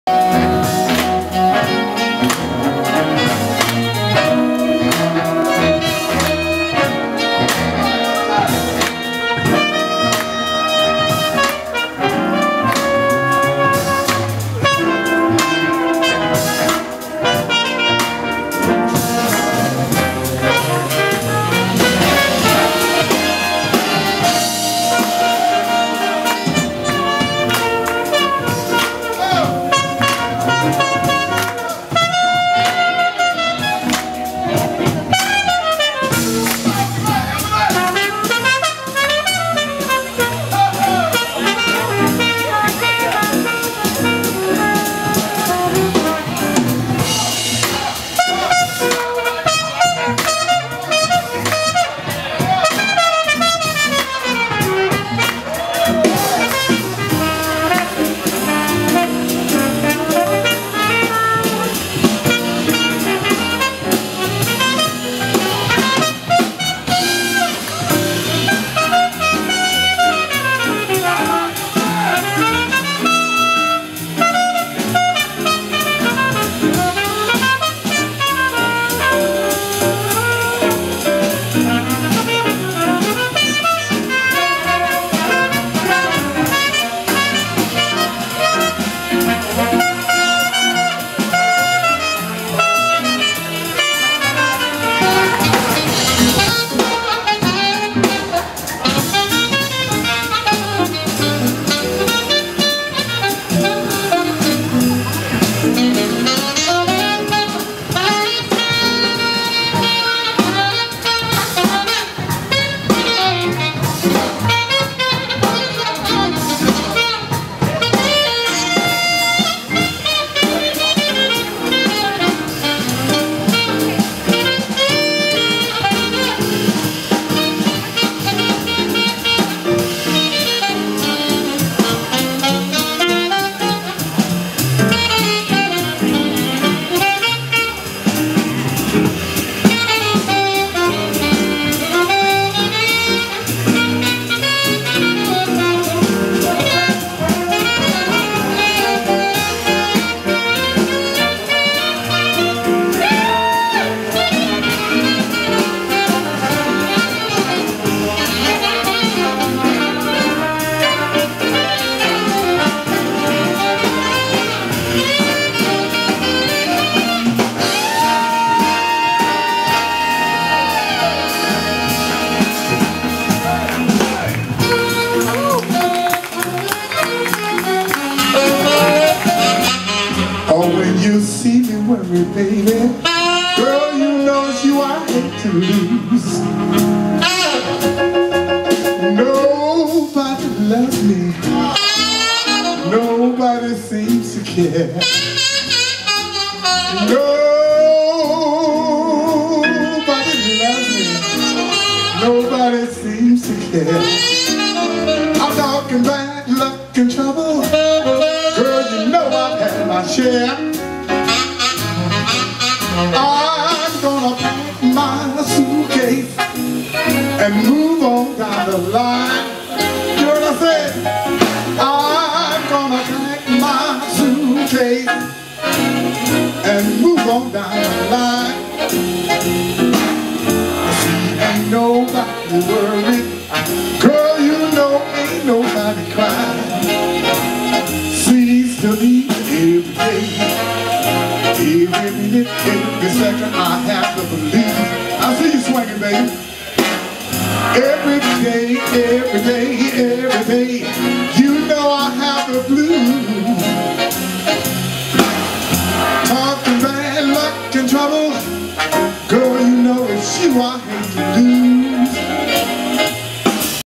DCA: All-American College Band with guest Ron Carter
Friday, June 24, 2016 - A couple of clips of the 2016 Disneyland Resort All-American College Band performing on the Hollywood Backlot Stage with guest Ron Carter.